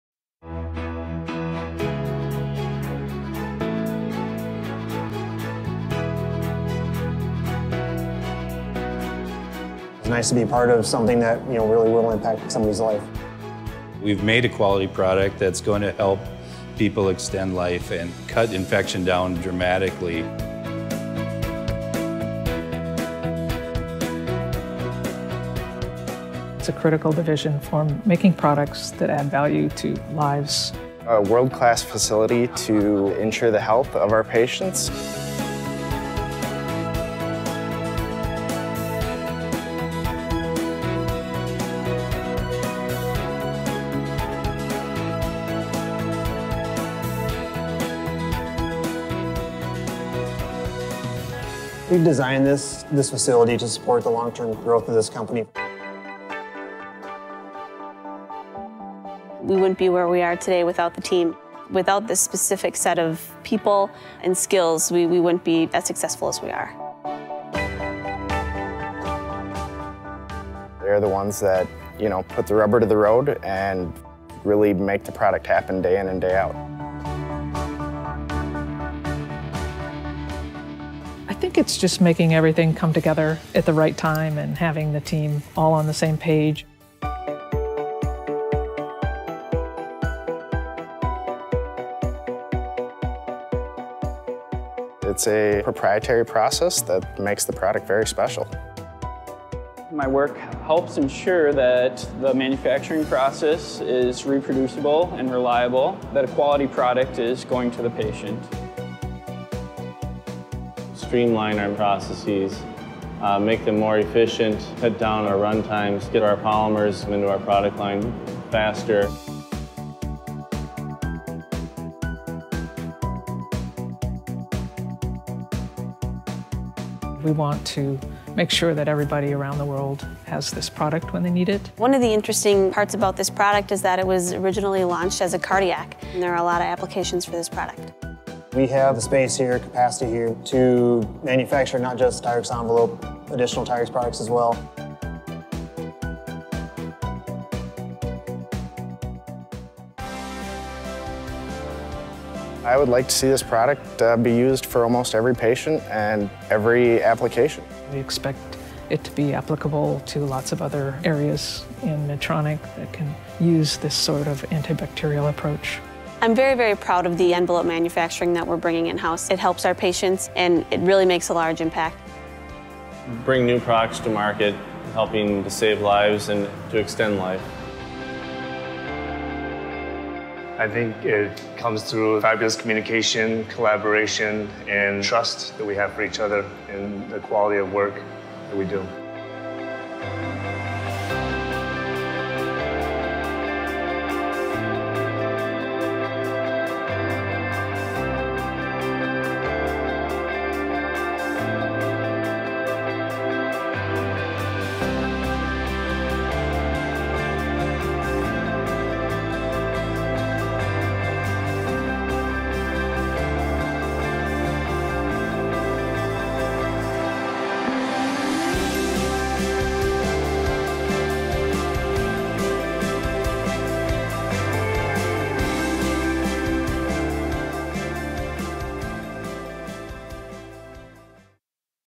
See inside the Medtronic TYRX™ Rice Creek Research, Development, & Manufacturing Facility
Medtronic TYRX™ Absorbable Antibacterial Envelope at the Rice Creek facility. World class technology, people, & passion goes into making it.